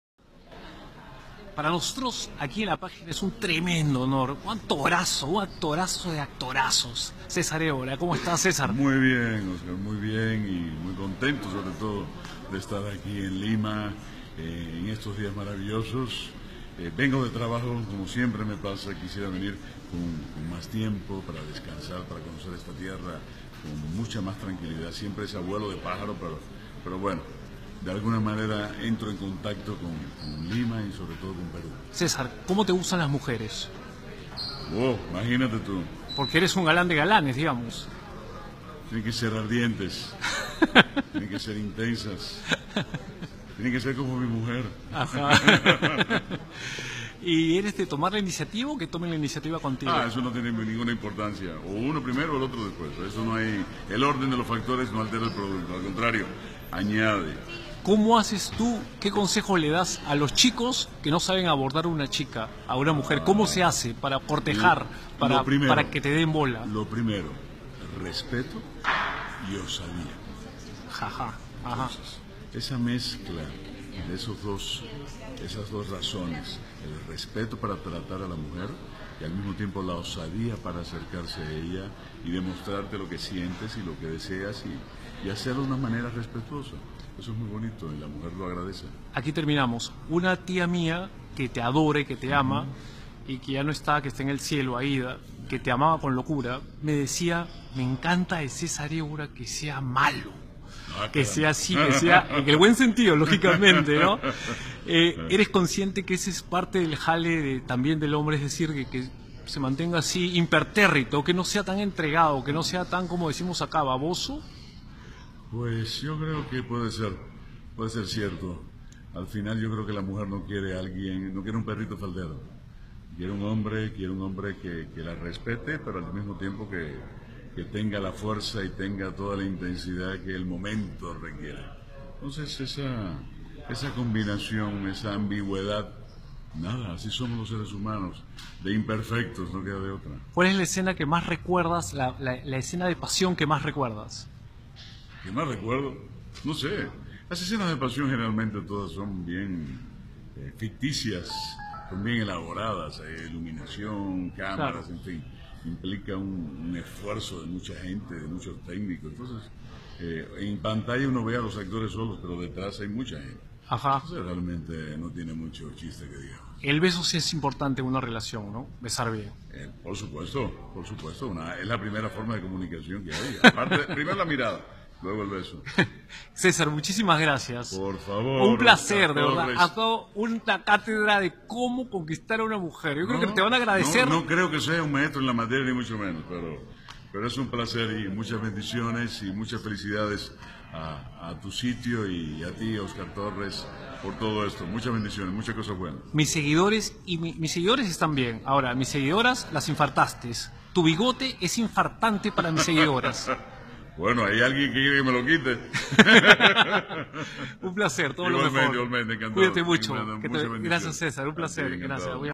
¿Como le gustan las mujeres a Cesar Evora?
Entrevista exclusiva para la página de Oscar Torres nuestro cubano CharlaEntreAmigos ‪#‎México‬ ‪#‎Televisa‬ ‪#‎periodismo‬ ‪#‎exclusivo‬ ‪#‎Perú‬